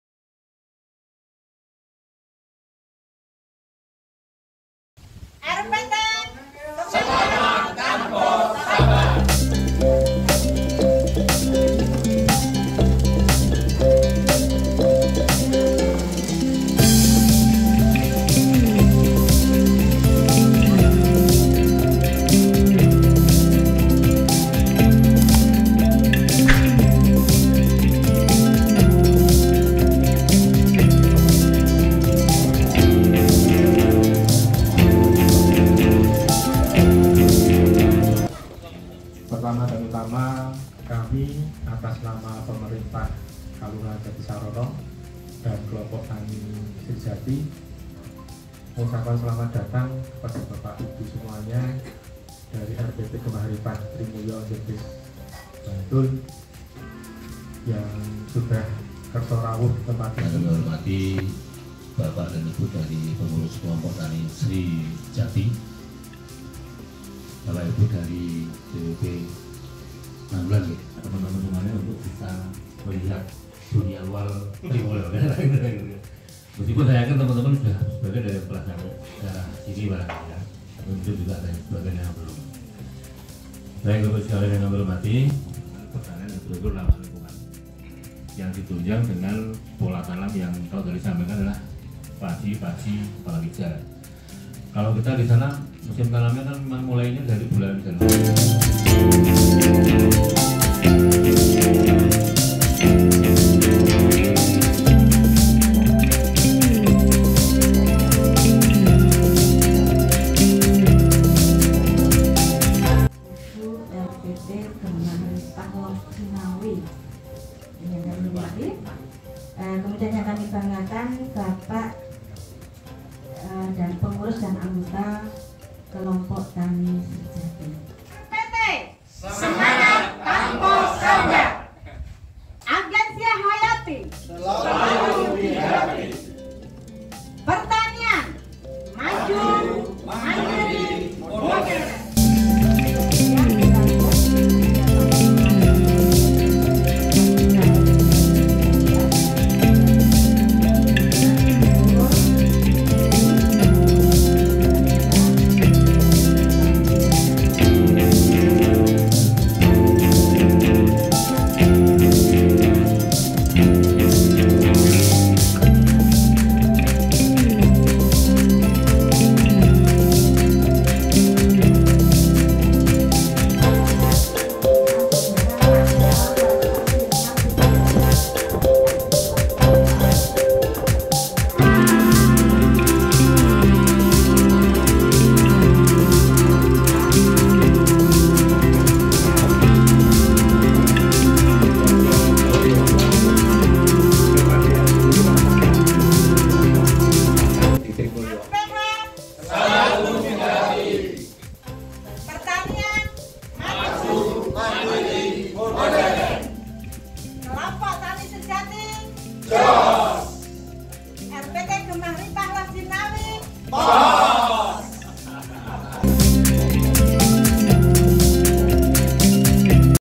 Ngangsu Kawruh Pertanian ramah lingkungan dan berkelanjutan
Ngangsu Kawruh Pertanian ramah lingkungan dan berkelanjutan RPT Gemah Ripah Trimulyo Sanden Bantul di Kelompok Tani Srijati Bejaten Jatisarono Nanggulan KulonProgo